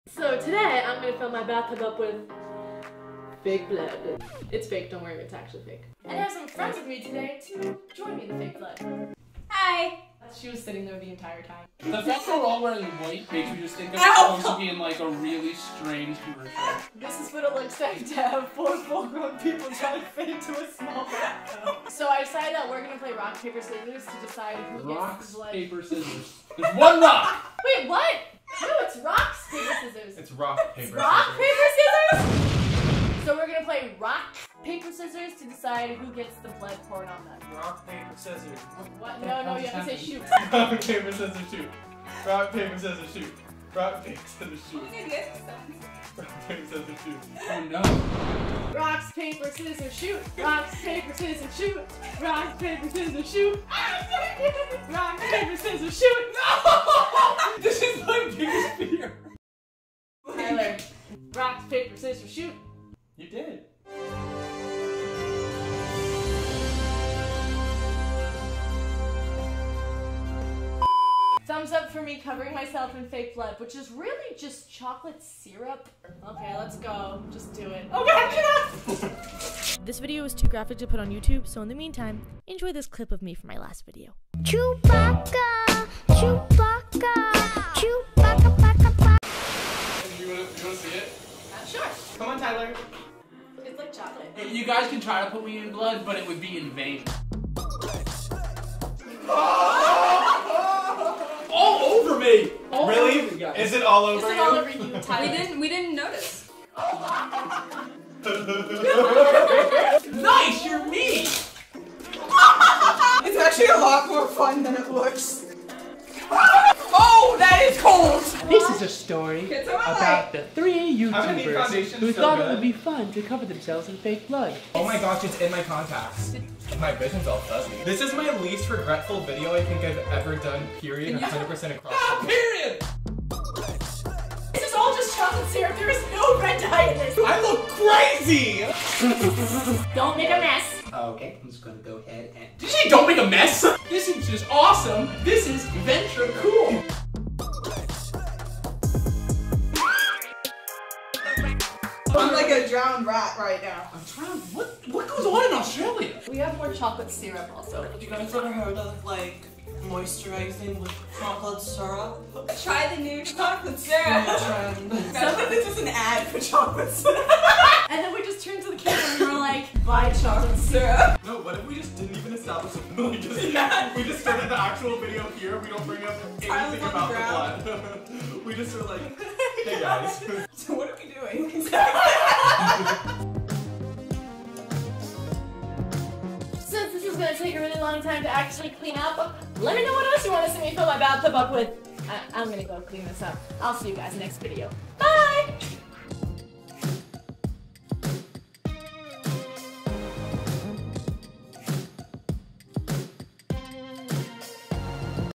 50 Gallons Of Fake Blood in Bathtub! (Halloween Bath Challenge)
I DECIDED TO do a fake blood bath challenge, I normally put weird things in the hot tub, like 10 million orbeez in hot tub, or 5000 gallons of slime in hot tub, but now we're moving onto the bathtub in spirit of halloween, I hope you enjoy

Want to send me something?
Rachel Levin
Philadelphia, PA
19102